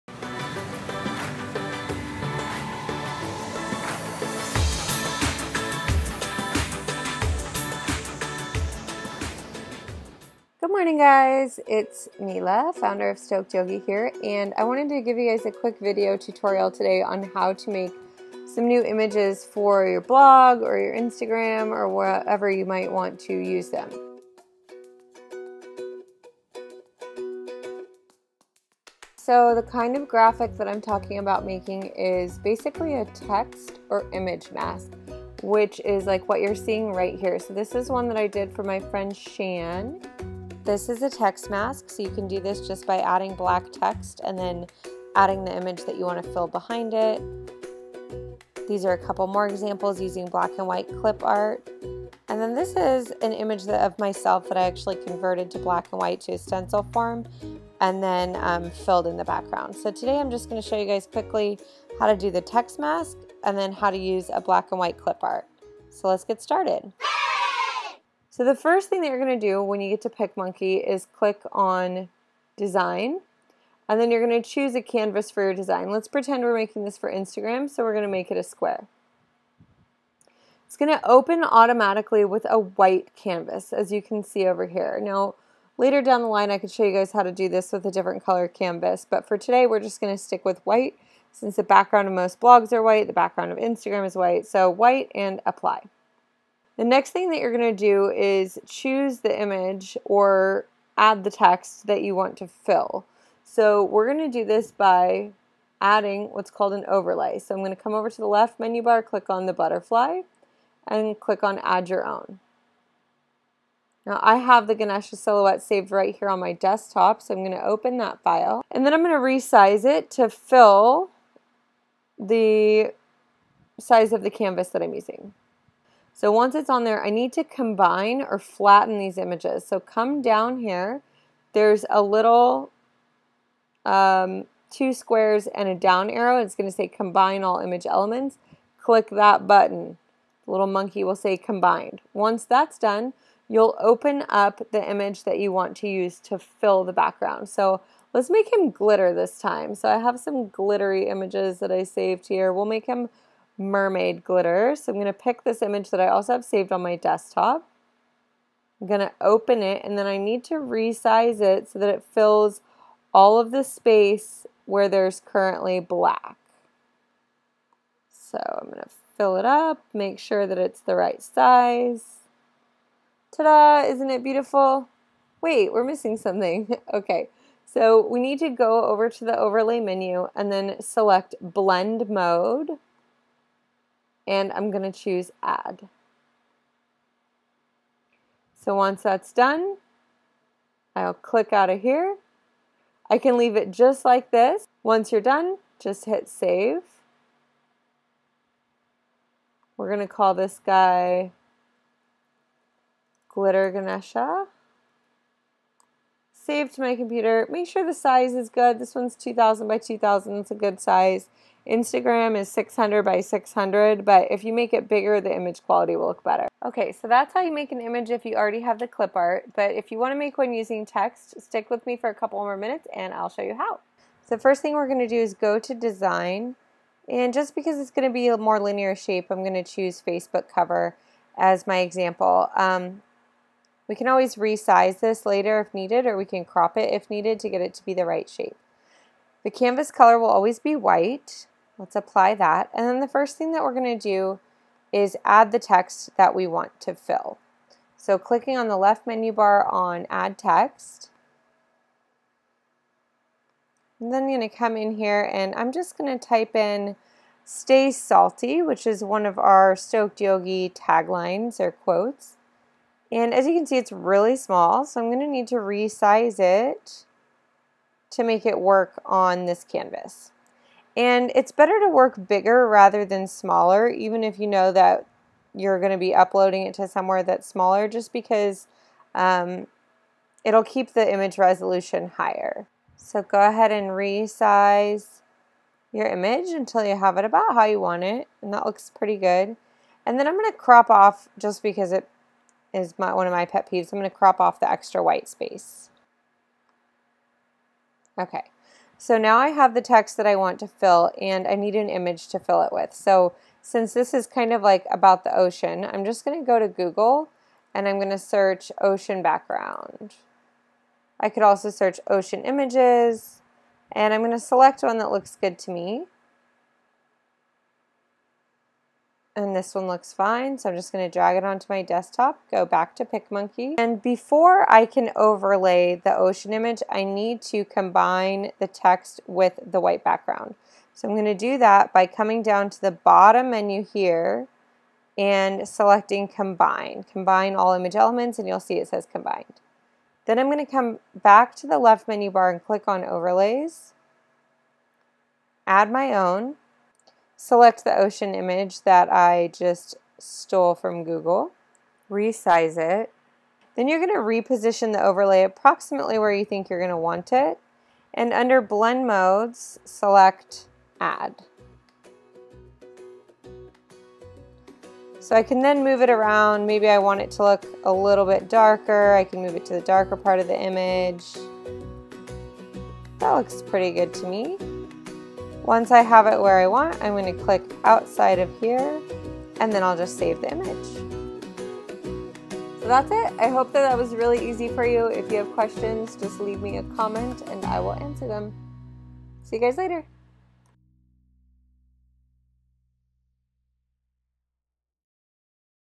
How to Make Text and Image Masks in PicMonkey
WANNA SKIP TO THE GOOD STUFF?!
Image Mask Tutorial @ 1:15
Text Mask Tutorial @ 3:50

This tutorial shows you a super easy way to spice up your blog, Pinterest, YouTube vids or Instagram with text mask images. You can use PicMonkey (which is FREE) to do the same graphic design techniques as Photoshop or Illustrator - in a fraction of the time.

I'll take you step by step through how to make a text or image mask in PicMonkey with existing black and white clipart, or any text phrase or word you want to use.

You can also check this out on the blog at stokedyogi.com

STAY SOCIAL